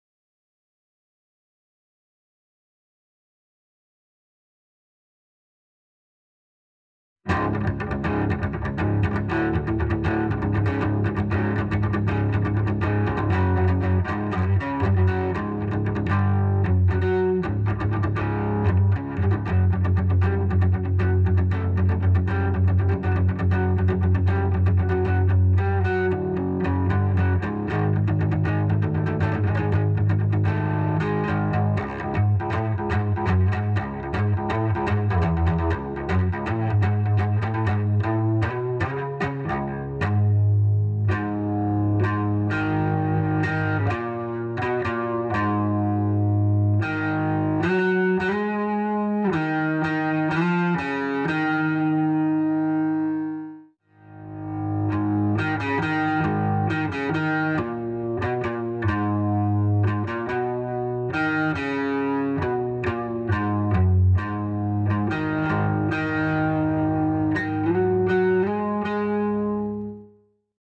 Cello simulator (Apocalyptica Style) with Electric Guitar and Helix
Cello sound (Apocalyptica Style) played with an Electric Guitar (Fender Strato, Neck Pickup) and Helix special preset and not free customized by me IR.
Standard pick (no bow or Ebow). Maybe I could use an Ebow, next time.